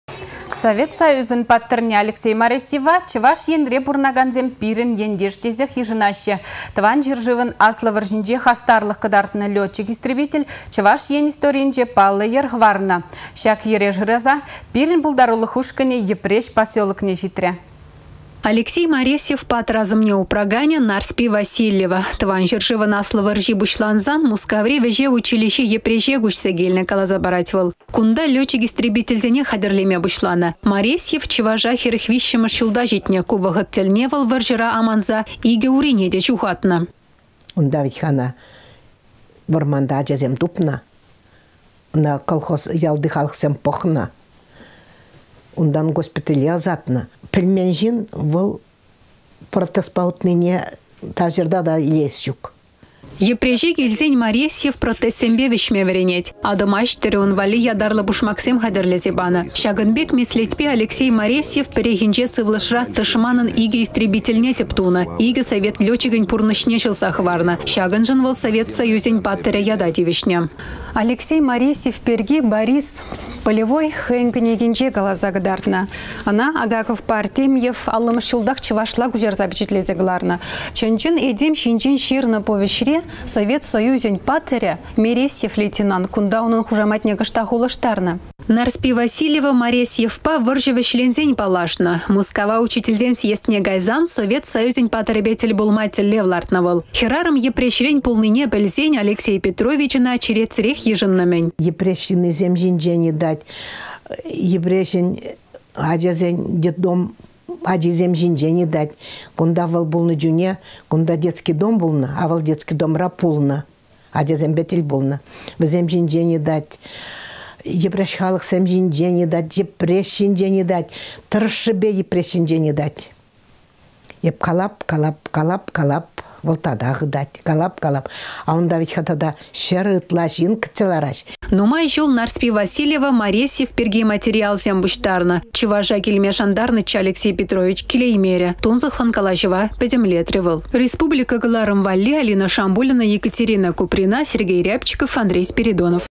Маресьев
Совет Союзĕн Паттăрне Алексей Маресьева Чăваш Енре пурăнакансем пирĕн ентеш тесех йышăнаççĕ. Тăван çĕршывăн аслă вăрçинче хастарлăх кăтартнă летчик-истребитель Чăваш Ен историйĕнче паллă йĕр хăварнă. Çак йĕре шыраса пирĕн пултарулăх ушкăнĕ Йĕпреç поселокне çитрĕ.